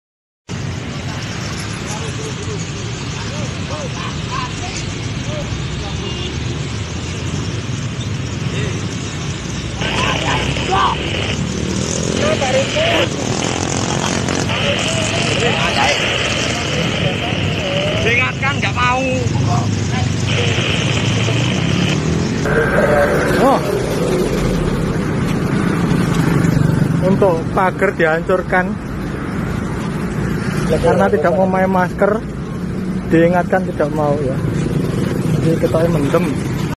Seorang Pria Yang Kedapatan Tidak Menggunakan Masker Dan Diduga Mabuk Merusak Pos PPKM
Video lengkap seorang pria yang kedapatan tidak menggunakan masker dan diduga mabuk merusak pos PPKM di Candi Sidoarjo.

Pria ini kemudian diamankan oleh anggota Babinsa dibantu warga setempat.